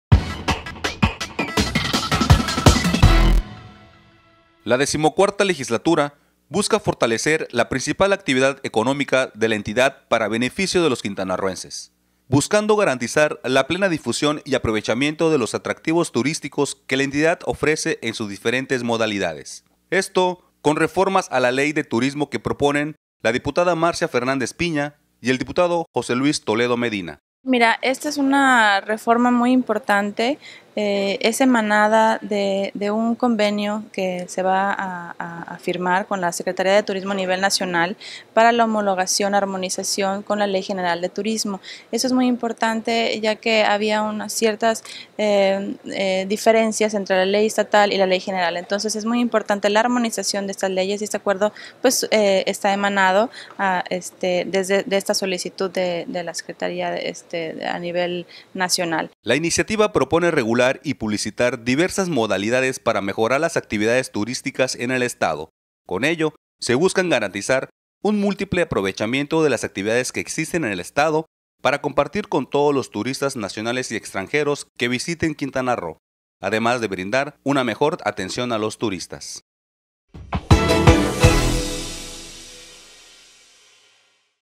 Promueven iniciativas para fortalecer al turismo en Quintana Roo
El Congreso del Estado dio entrada a una serie de reformas estratégicas con las que se busca garantizar la plena difusión y aprovechamiento de los atractivos turísticos de la entidad en sus diferentes modalidades, así como fortalecer la principal actividad económica de la entidad para beneficio de los quintanarroenses.